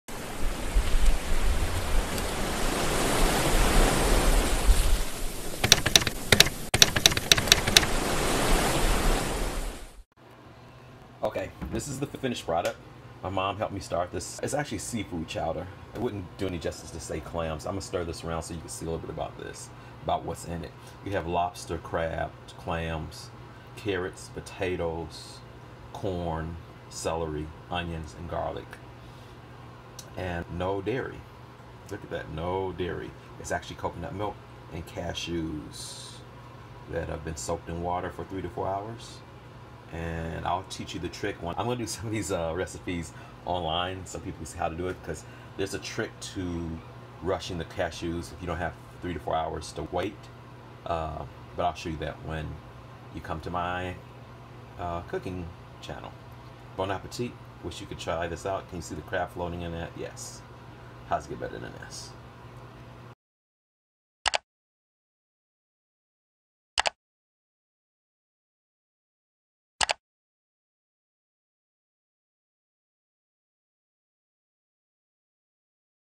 Orgasmic Conscious Cooking - Dairy-less Seafood Chowder
Inspired by my Mom! This is my dairy-less seafood chowder!  Instructional video and recipe coming soon!!